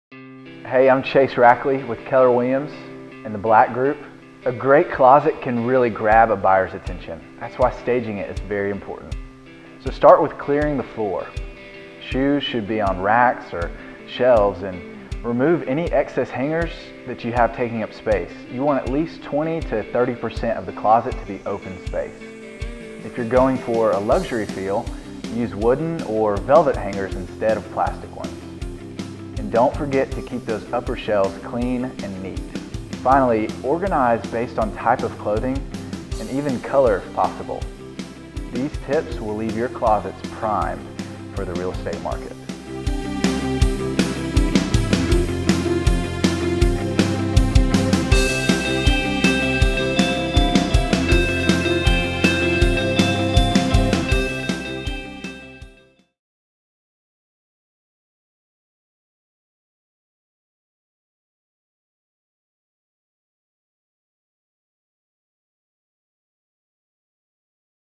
Clean Out Your Closet
A great closet can really grab the buyers attention. That's why staging it is very important. You want at least 20-30% of the closet to be open space. These tips will leave your closets primed for the real estate market.